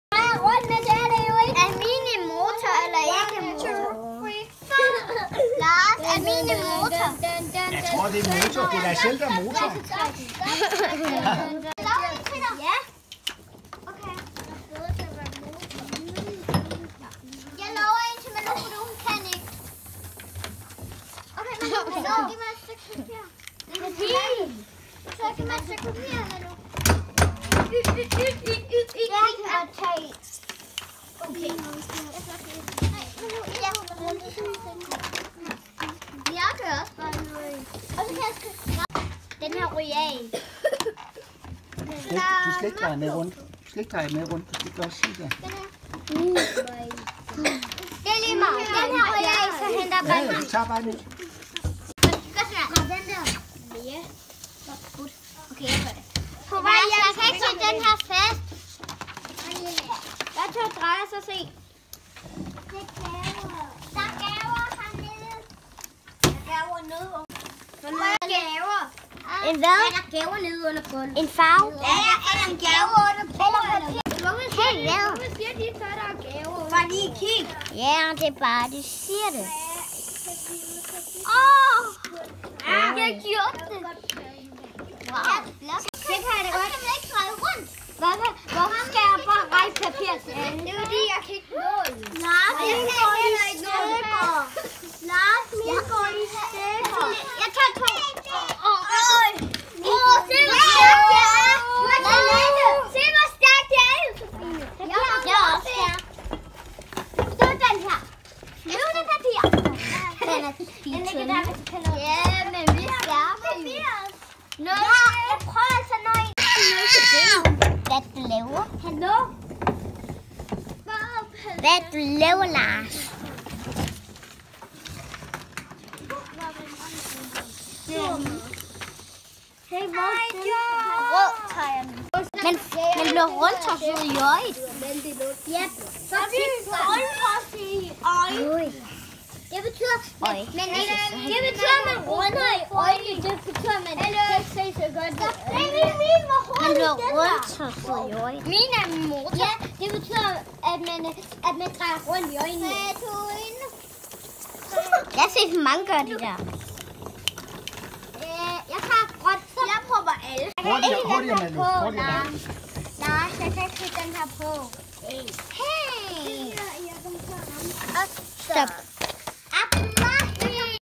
Du bliver rundtosset i øjnene - Snurretegning 0 kl 2020
Tag en kavalet til ler. Placer en lille klat elefantsnot i centrum. Sæt et A4-papir fast. Snur hurtigt rundt og tegn helst med tush. ADVARSEL: Du bliver rundtosset i øjnene :)